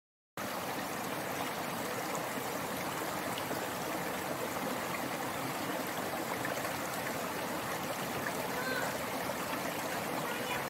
SUNRISE in winter
The sun rise is magnificent and the stream from the natural hotspring is so relaxing!

The Baths
Citrusdal

Please subsribe for more random events.